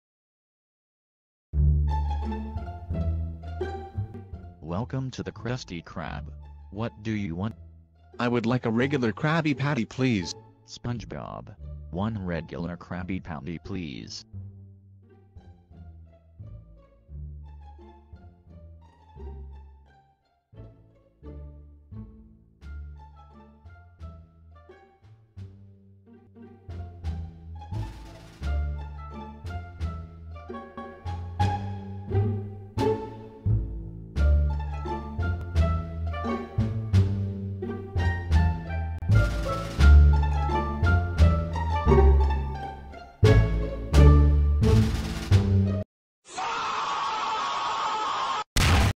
Spongebob and The Last Krabby Patty
This is my first 3D animation using Garry's Mod AKA GMOD